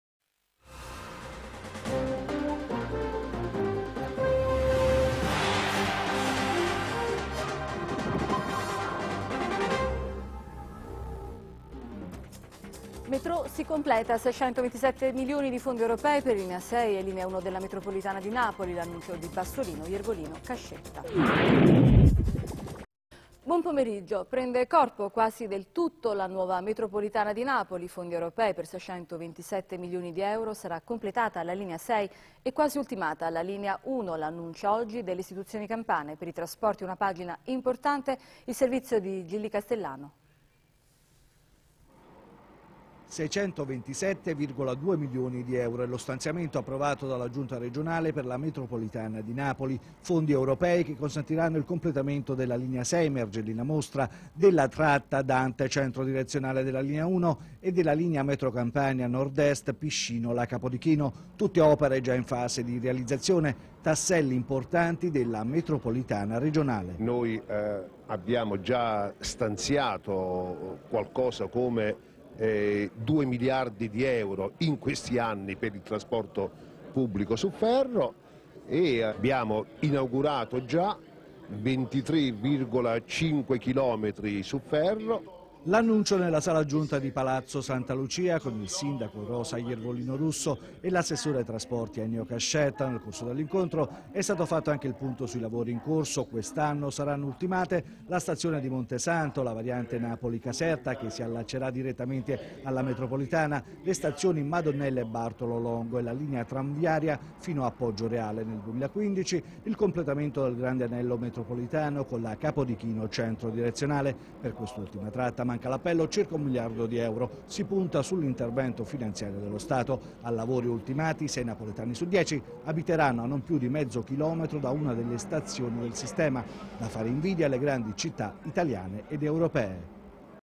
Finanziamenti Metropolitana di Napoli
Servizio del TGR sui finanziamenti alla Metropolitana di Napoli concessi dalla Regione Campania.